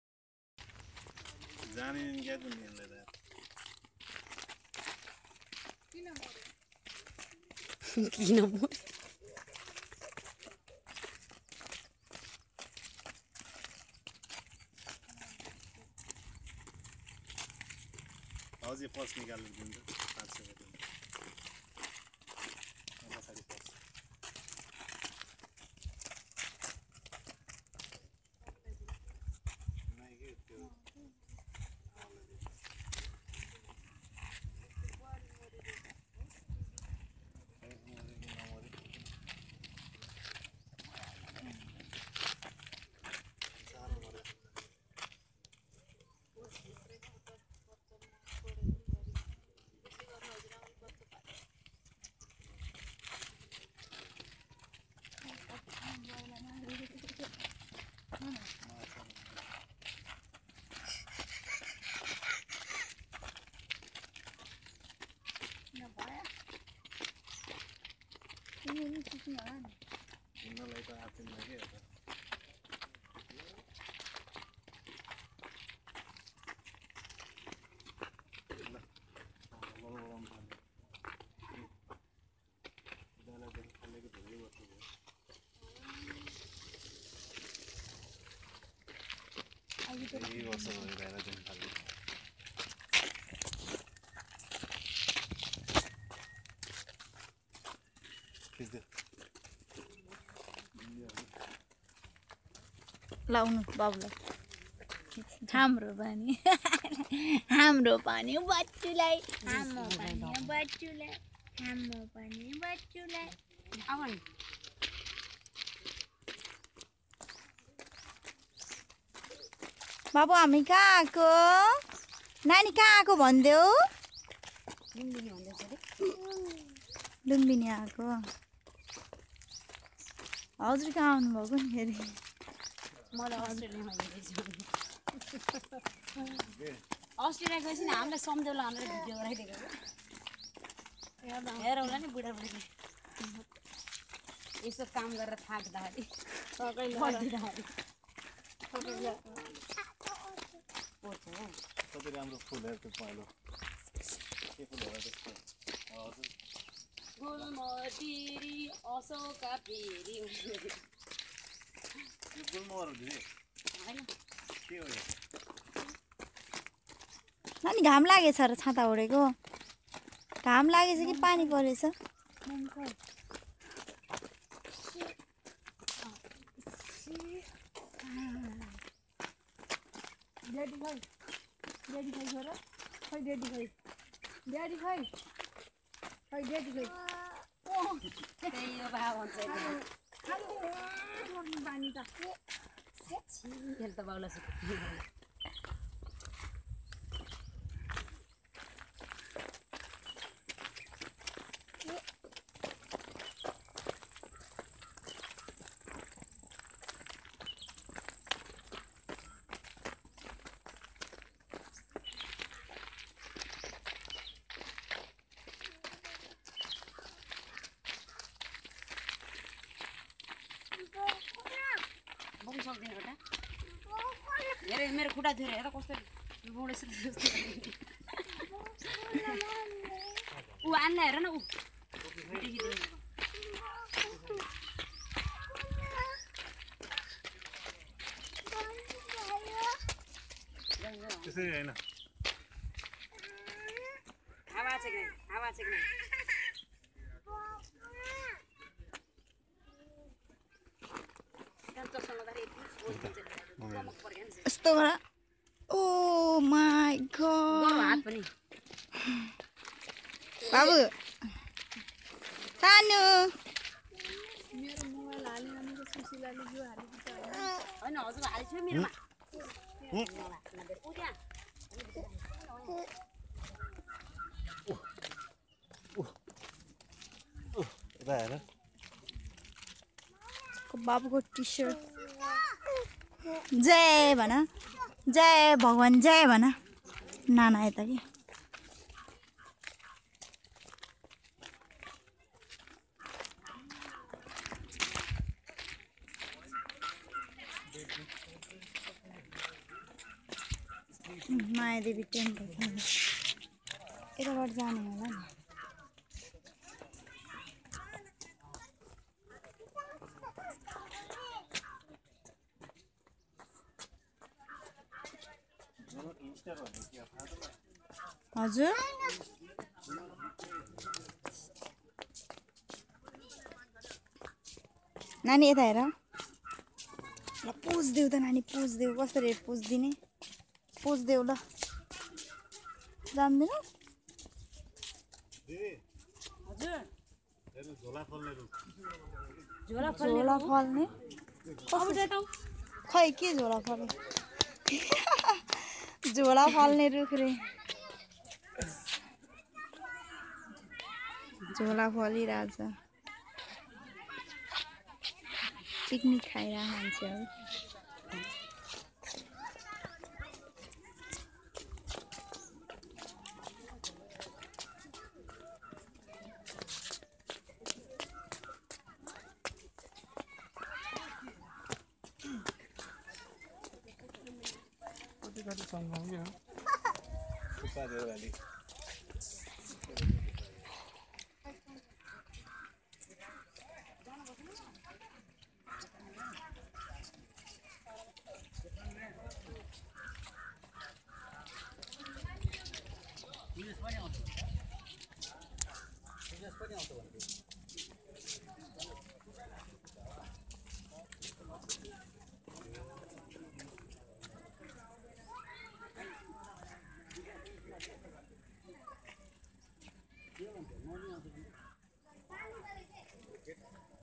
LUMBINI NEPAL-BIRTH PLACE OF GAUTAM BUDDHA
This video clip is all about the family visit to Lumbini it was taken in 2016 .the best part of the video is the activities of adorable PRAGYAN.